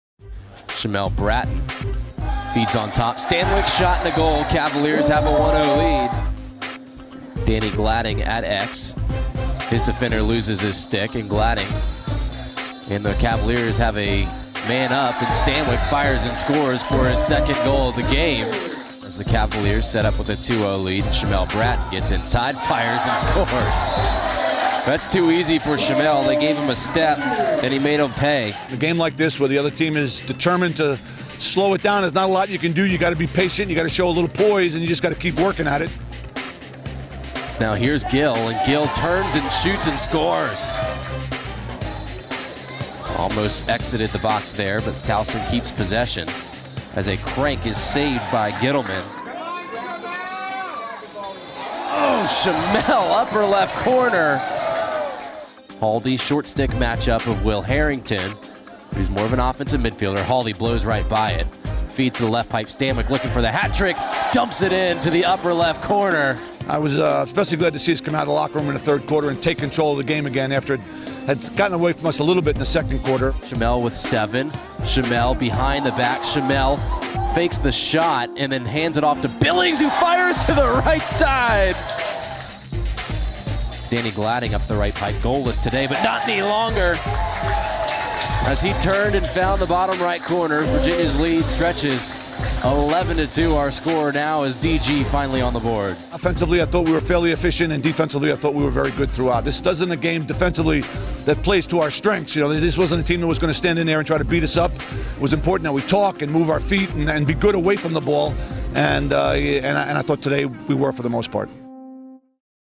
Men's Lacrosse Highlights - Towson
#1 Virginia handles Towson, 11-2.